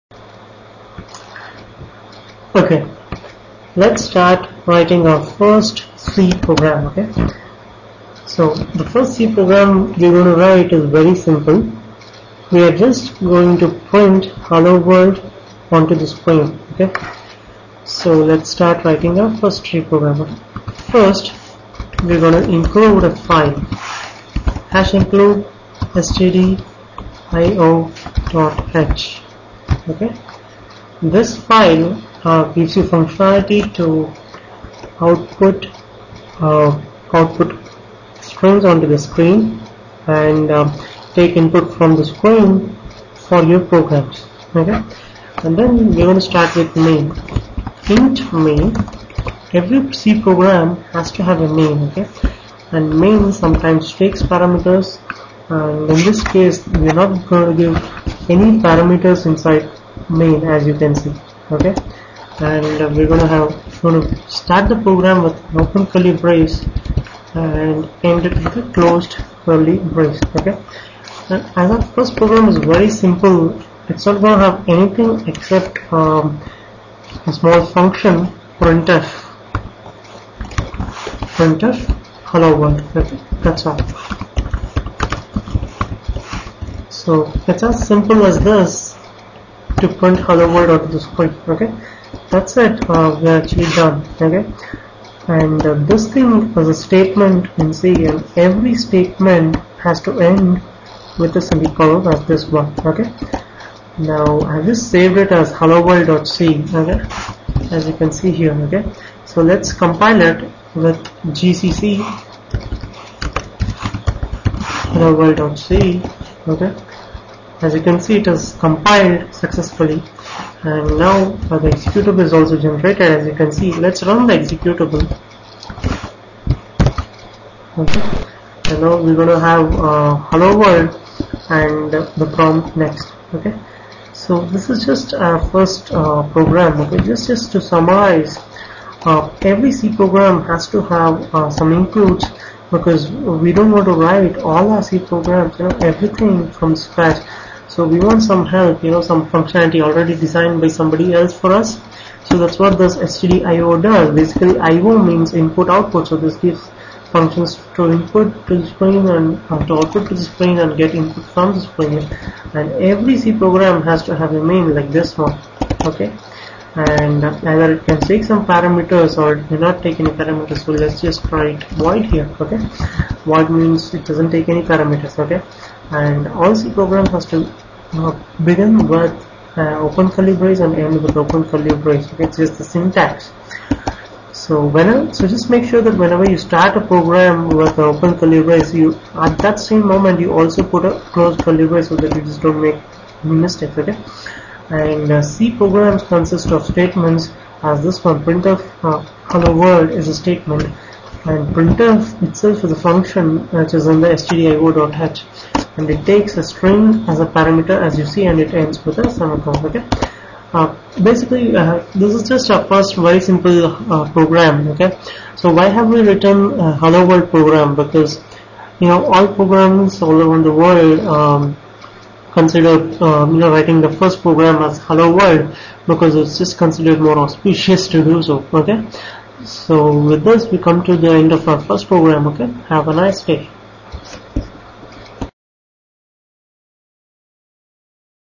Programming in C : Lesson 1
Our first C program - HelloWorld program.
It prints the string "Hello World" to the screen and introduces C as a programming language